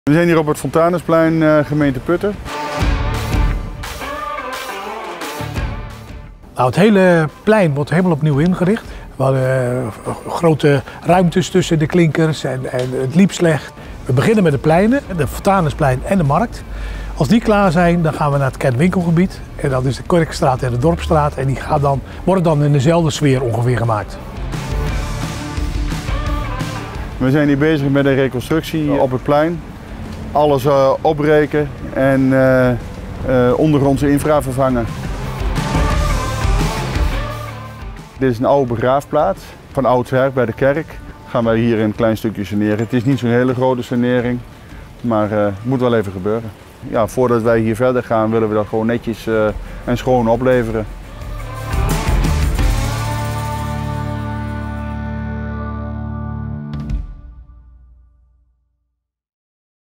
Herinrichting centrum Putten door Van Gelder | Hoe gebeurt dat?
De herinrichting van het centrum in Putten. Hoe doen we dat? Uitvoerder Henri Struik vertelt het je in deze video.